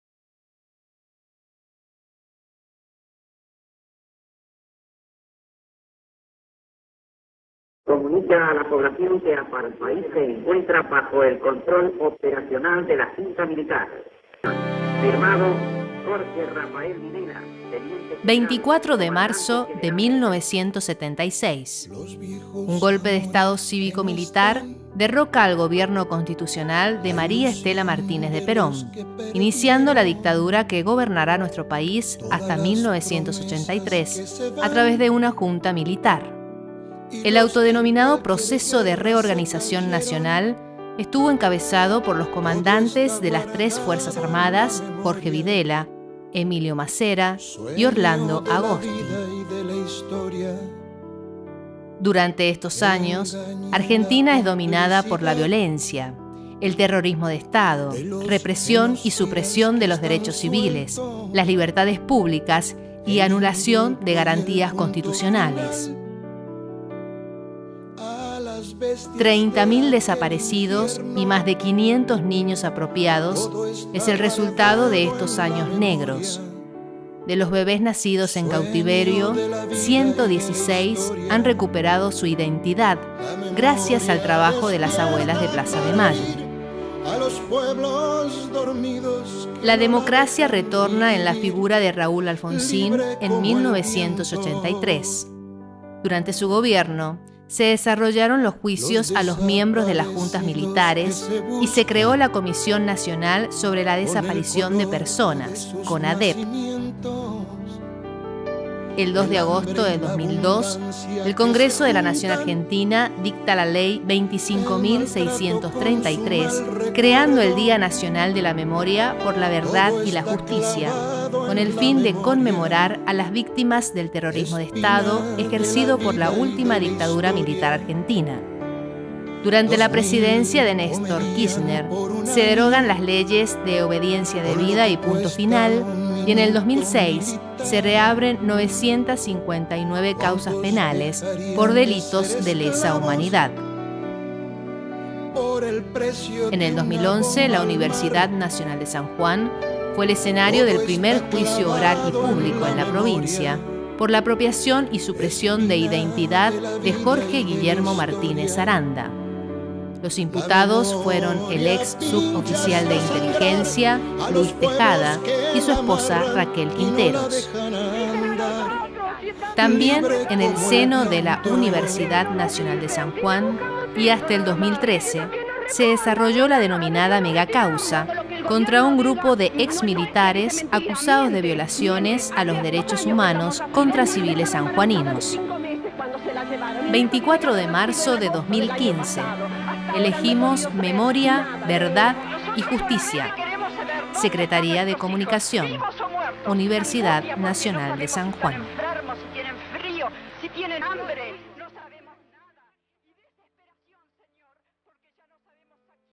24 de Marzo: Memoria, Verdad y Justicia- Universidad Nacional de San Juan
Día Nacional de la Memoria, por la Verdad y la Justicia 2015

Secretaría de Comunicación- UNSJ